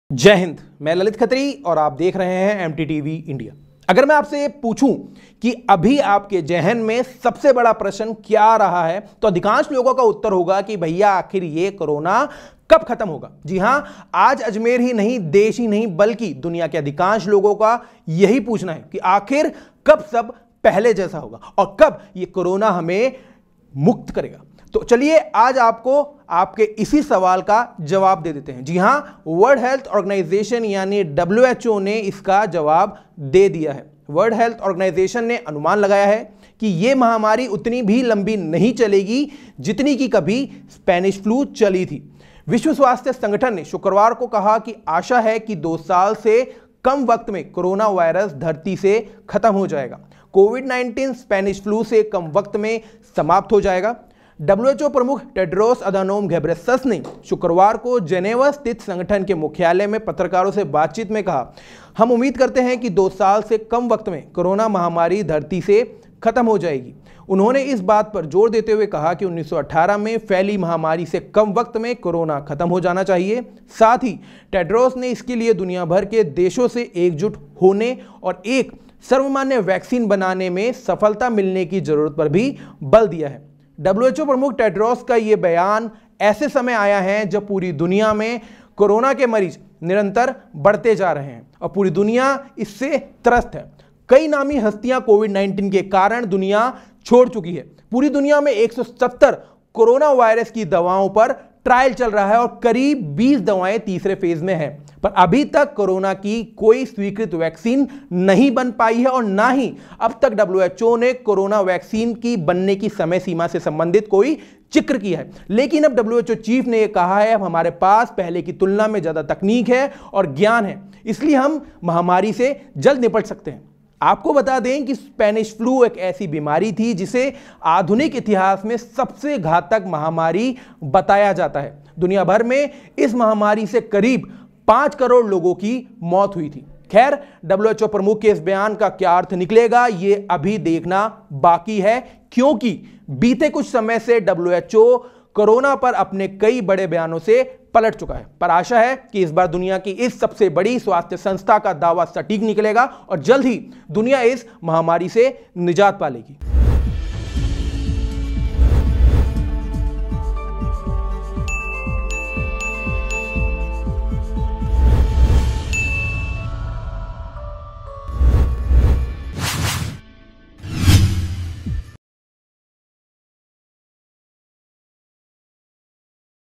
WHO का दावा कोरोना महामारी दो साल के भीतर हो जाएगी खत्म| MTTV INDIA
WHO का दावा कोरोना महामारी दो साल के भीतर हो जाएगी खत्म,WHO ने कोरोनावायरस का स्पेनिश फ्लू की तुलना में कम समय तक रहने का अनुमान लगाया ,इतिहास में स्पेनिश फ्लू को सबसे घातक महामारी बताया गया था